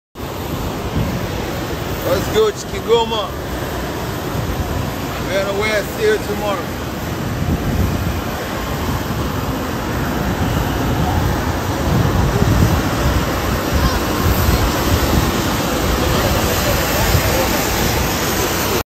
MCHEKI KONDEBOY JESHI ALIVYOTUA KIGOMA NYUMBANI WA VIPAJI
Harmonize arriving at Kingoma home of talent